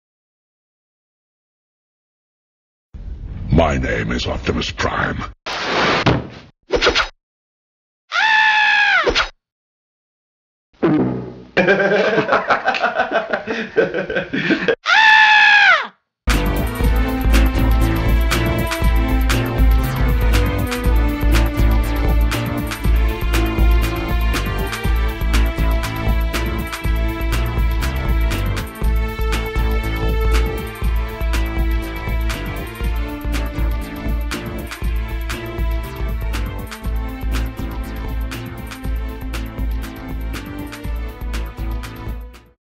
BIONICLE: Mata Nui's Quest of Knowledge: REALLY?! Takanuva Finds Allspark!
BIONICLE: Mata Nui's Quest of Knowledge is an awesome interactive game here on YouTube! Click on the answers to go to the next video and see if you go it right! HELP MATA NUI RESTORE BIONICLE'S HISTORY!